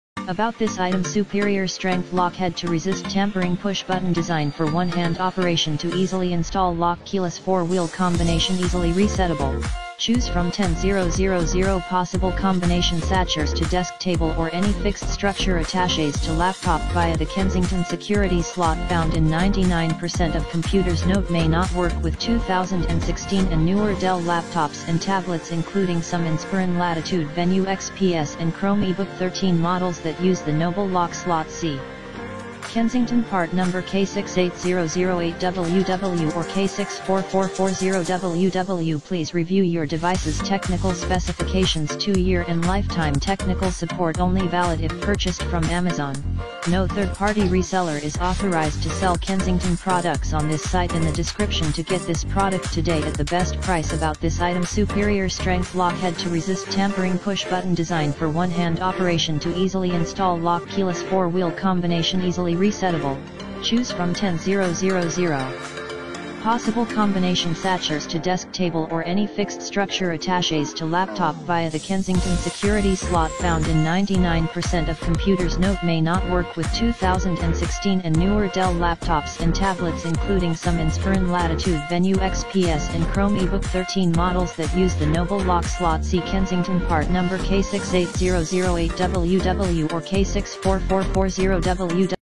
Kensington Combination Ultra Cable Lock for Laptops and Other Devices K64675US,Silver
Kensington COMBINATION ULTRA LAPTOP LOCK (K64675US)

*Superior strength lockhead to resist tampering
*Push button design for one hand operation to easily install lock Keyless 4-wheel combination
*Easily resettable - choose from 10 000 possible combinations
*Achors to desk table or any fixed structure Attaches to laptop via the Kensington Security Slot found in 99% of computers
*NOTE May not work with 2016 and newer Dell laptops and tablets including some Inspiron Latitude Venue XPS and Chromebook 13 models that use the Noble lock slot See Kensington part# K68008WW or K64440WW Please review your devices technical specifications
2 year and lifetime technical support only valid if purchased from amazon, no third party reseller is authorized to sell Kensington Products on this site

Bangkok Business equipment Automation Co., Ltd. Bangkok, Thailand